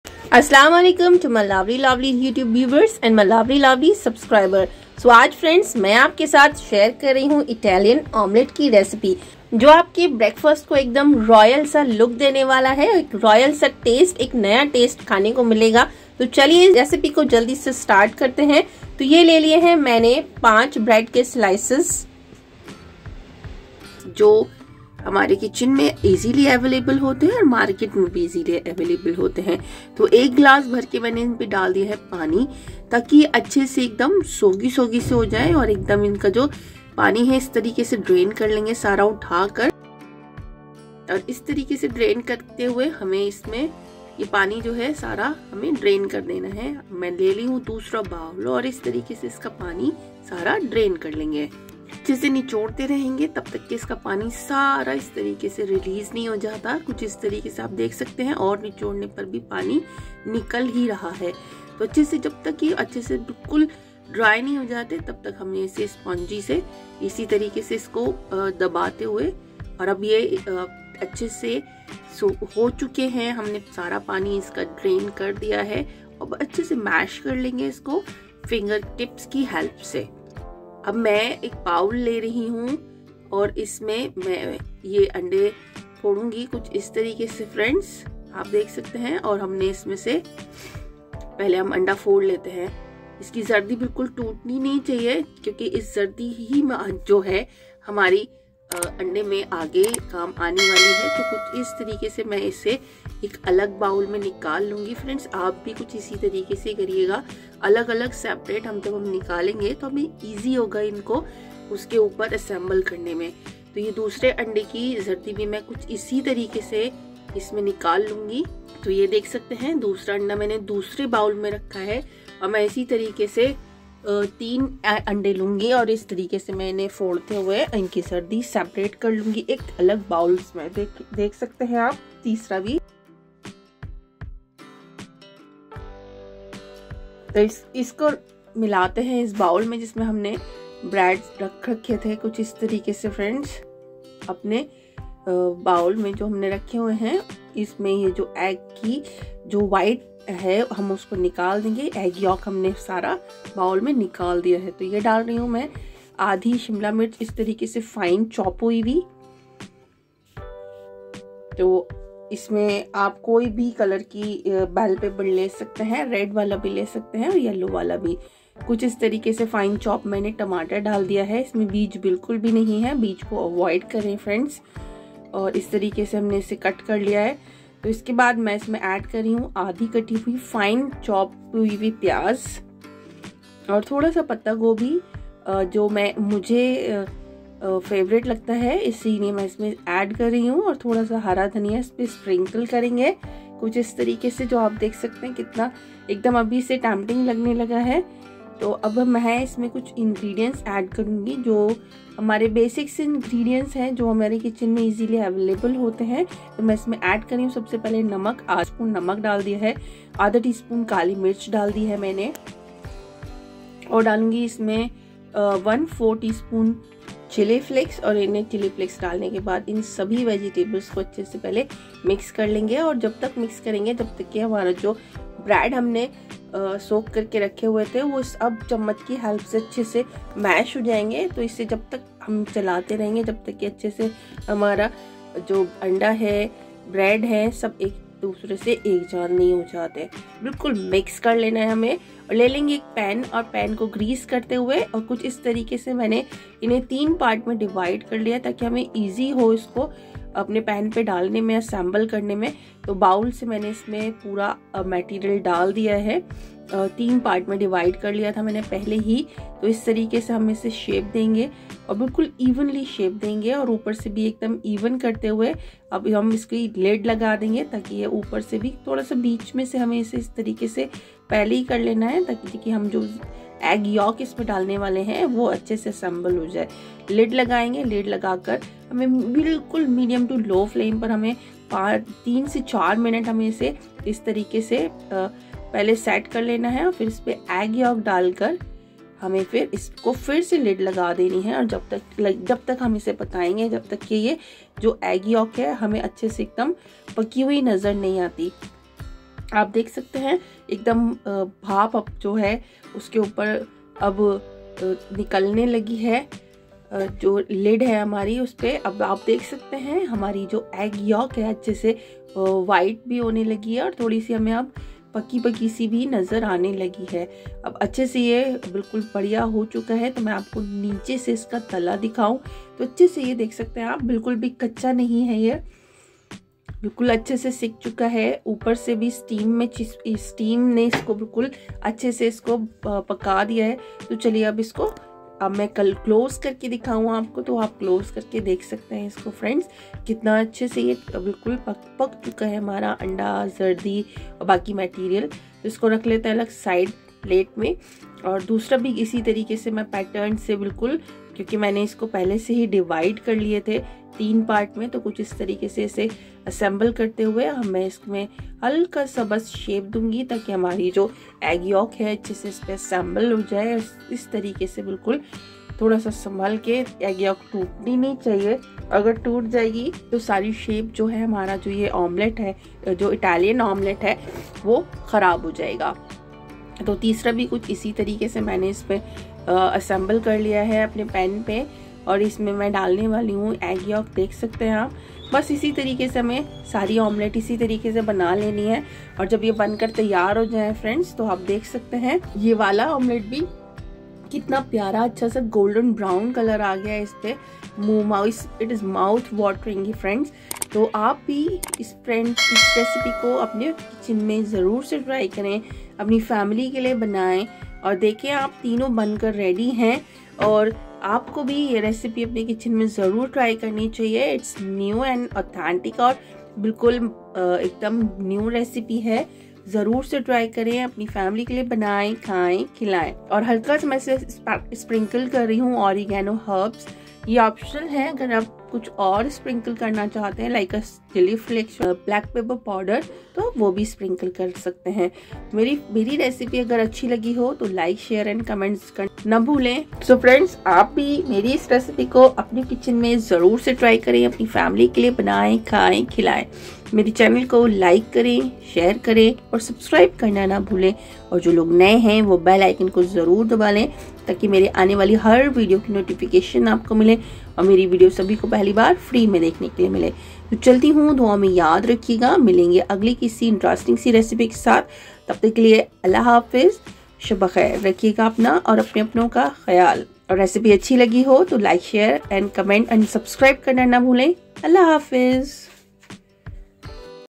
Italian Omelette and Eggs Recipe | Easy Italian Morning Breakfast Delight - with a Twist...
Today, I'm taking on the challenge of making the perfect Italian breakfast with a twist—let's see if I can whip up my version of eggs and fresh vegetables that will blow your mind!

Start your day with a delicious and easy-to-make Italian omelette recipe! This breakfast favorite is packed with flavorful ingredients and is ready in just 20 minutes. In this video, we'll show you how to make a mouthwatering Italian omelette filled with, savory ham, and fresh vegetables. Perfect for busy mornings or a weekend brunch, this recipe is a must-try for anyone who loves Italian cuisine. Get ready to elevate your breakfast game and impress your family and friends with this simple yet impressive dish.

Ingredients:- 👇
5 Bread Slices
3 Eggs
½ Fine Chopped Capsicum
1Chopped Tomato
½ Chopped Onion
Fresh Coriander
1tspn Salt
1tspn black papper
¼tspn Chilli Flakes
Oil or butter as needed

HF - 550 Tripot